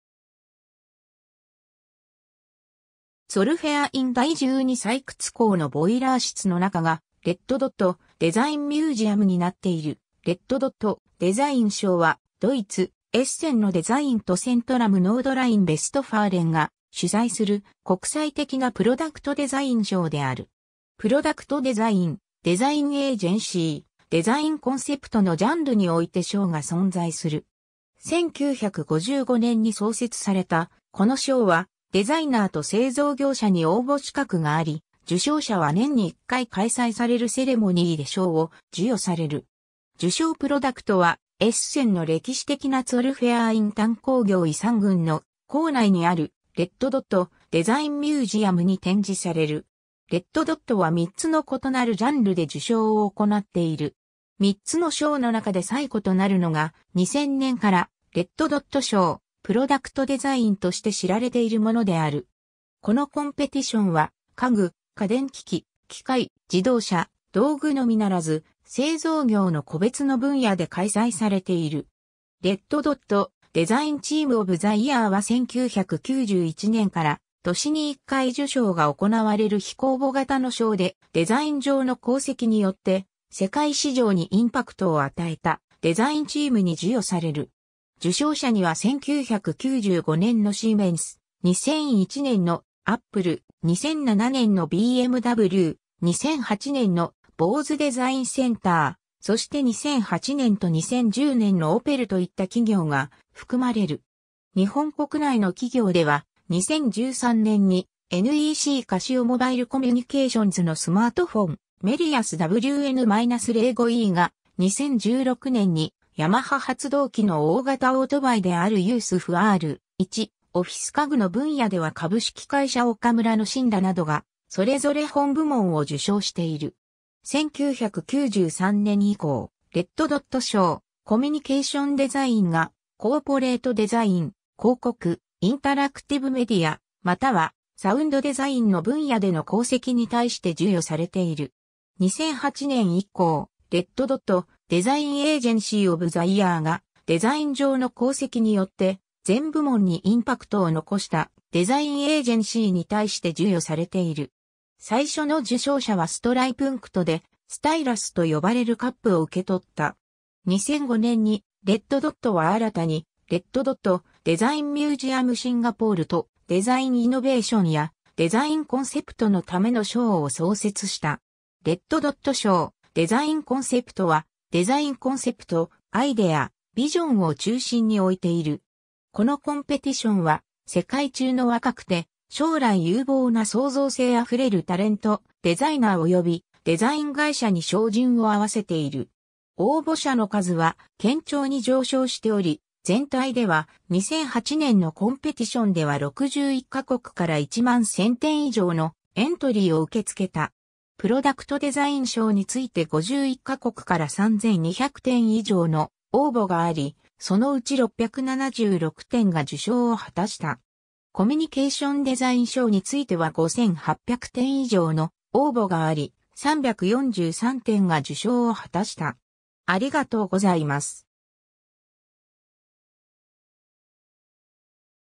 レッド・ドット・デザイン賞
ツォルフェアアイン第12採掘坑のボイラー室の中がレッド・ドット・デザイン・ミュージアムになっている レッド・ドット・デザイン賞（レッド・ドット・デザインしょう、red dot design award）は、ドイツ、エッセンのDesign Zentrum Nordrhein Westfalen（ノルトライン＝ヴェストファーレンデザインセンター）が主催する国際的なプロダクトデザイン賞である。
プロダクトデザイン、デザインエージェンシー、デザインコンセプトのジャンルにおいて賞が存在する。
1955年に創設されたこの賞はデザイナーと製造業者に応募資格があり、受賞者は年に1回開催されるセレモニー (Ceremony) で賞を授与される。
受賞プロダクトはエッセンの歴史的なツォルフェアアイン炭鉱業遺産群の構内にある「レッド・ドット・デザイン・ミュージアム」に展示される。
 レッド・ドットは3つの異なるジャンルで授賞を行っている。
 3つの賞の中で最古となるのが、2000年から「レッド・ドット賞：プロダクトデザイン」(red dot award: product design) として知られているものである。
このコンペティションは家具、家電機器、機械、自動車、道具のみならず、製造業の個別の分野で開催されている。
 「レッド・ドット：デザインチーム・オブ・ザ・イヤー」(red dot: design team of the year) は1991年から年に1回授賞が行われる非公募型の賞で、デザイン上の功績によって世界市場にインパクトを与えたデザインチームに授与される。
受賞者には1995年のシーメンス、2001年のApple、2007年のBMW、2008年のボーズ・デザインセンター、そして2008年と2010年のオペルといった企業が含まれる。
 日本国内の企業では、2013年にNECカシオ モバイルコミュニケーションズのスマートフォン「MEDIAS W N-05E」が、2016年にヤマハ発動機の大型オートバイであるYZF-R1、オフィス家具の分野では株式会社オカムラのCYNARAなどがそれぞれ本部門を受賞している。
 1993年以降、「レッド・ドット賞：コミュニケーションデザイン」(red dot award: communication design) が、コーポレートデザイン、広告、インタラクティブ・メディアまたはサウンドデザインの分野での功績に対して授与されている。
 2008年以降、「レッド・ドット：デザインエージェンシー・オブ・ザ・イヤー」(red dot: design agency of the year) が、デザイン上の功績によって全部門にインパクトを残したデザインエージェンシーに対して授与されている。
最初の受賞者はStrichpunkt（ストリッヒプント）でStylus（スタイラス）と呼ばれるカップを受け取った。
 2005年に、レッド・ドットは新たに「レッド・ドット・デザイン・ミュージアム・シンガポール」と、デザインイノベーションやデザインコンセプトのための賞を創設した。
「レッド・ドット賞：デザインコンセプト」(red dot award: design concept) はデザインコンセプト、アイデア、ビジョンを中心に置いている。
このコンペティションは世界中の若くて将来有望な創造性あふれるタレント、デザイナーおよびデザイン会社に照準を合わせている。
 応募者の数は堅調に上昇しており、全体では、2008年のコンペティションでは61ヶ国から11,000点以上のエントリーを受け付けた。
プロダクトデザイン賞について51ヶ国から3,200点以上の応募があり、そのうち676点が受賞を果たした。
コミュニケーションデザイン賞については5,800点以上の応募があり、343点が受賞を果たした。